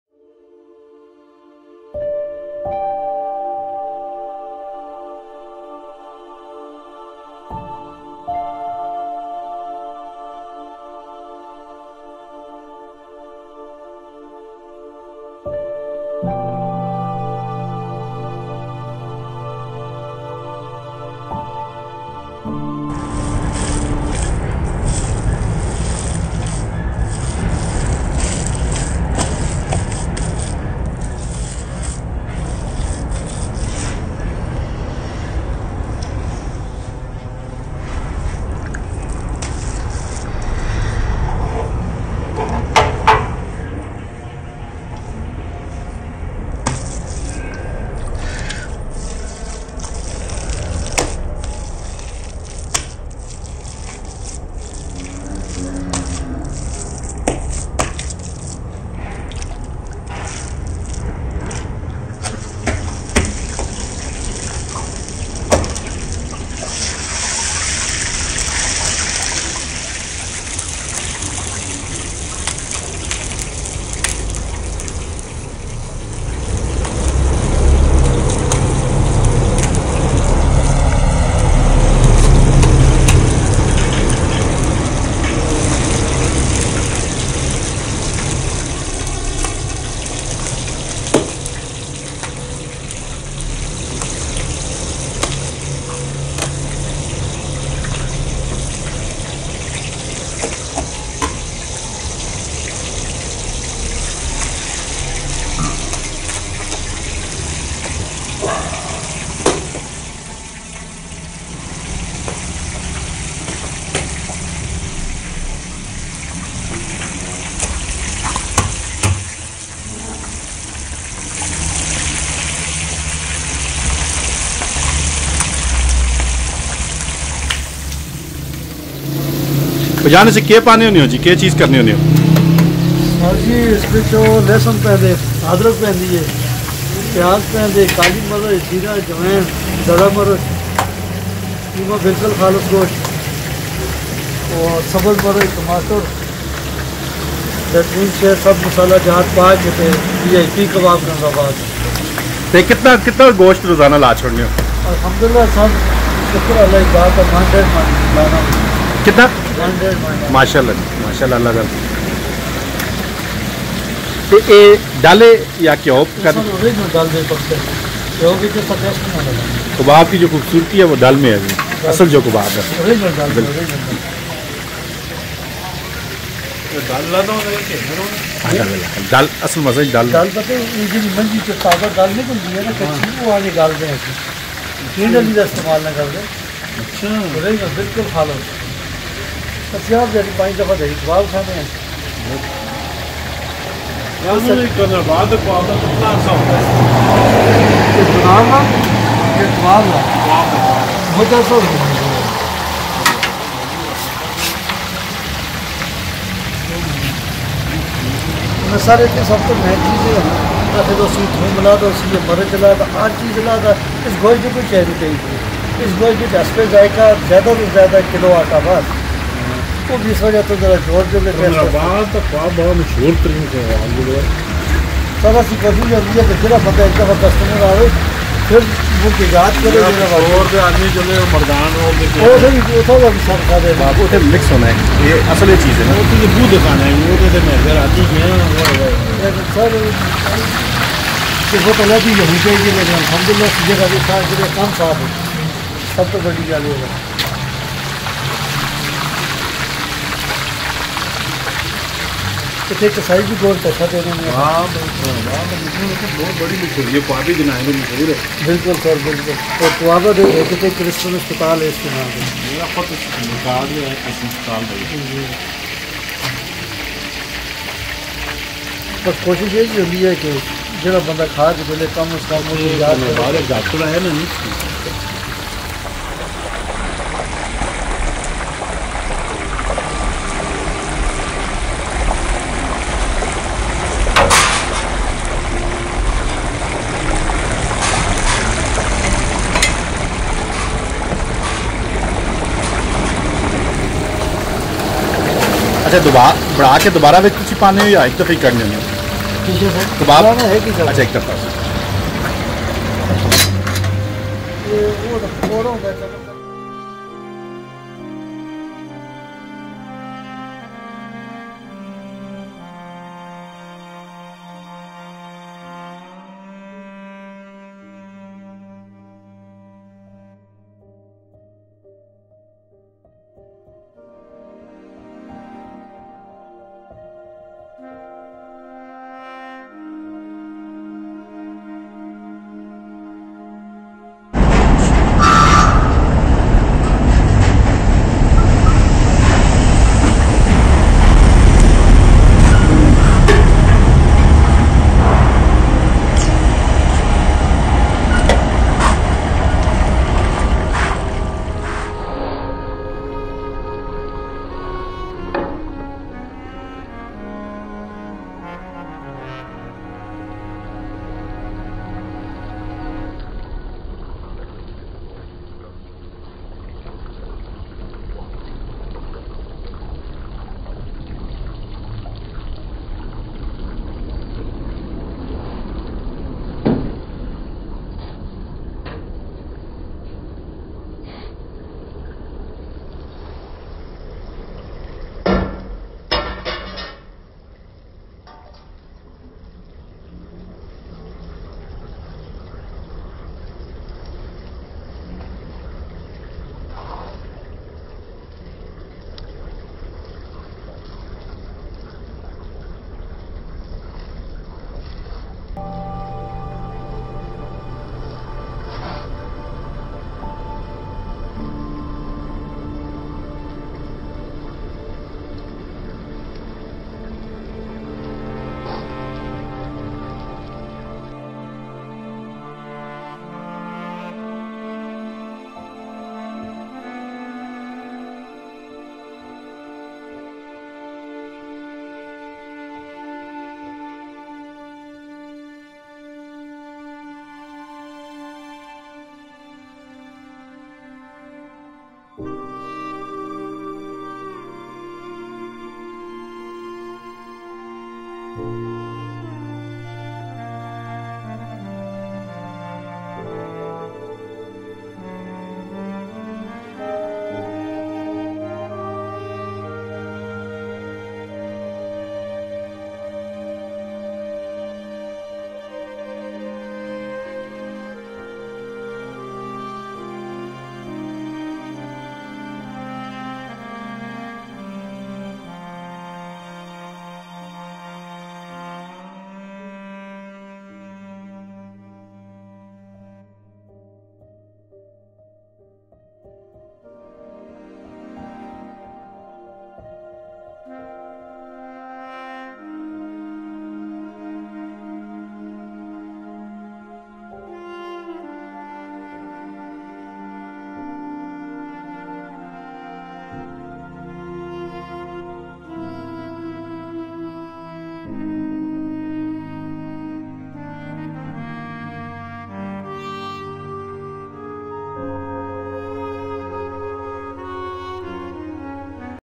Street Food | Famous Chapli Kababs of Qalandarabad | Abbotabad kpk Pakistan CPEC interchange
Chapli Kebab or Kabab ( چپلي کباب‎) is a Pashtun-style minced kebab, usually made from ground beef, mutton or chicken with various spices in the shape of a patty. It orginates from Afghanistan and can be found in Peshawar, also known in KPK as the Pekhawri Chapli Kabab or Peshawari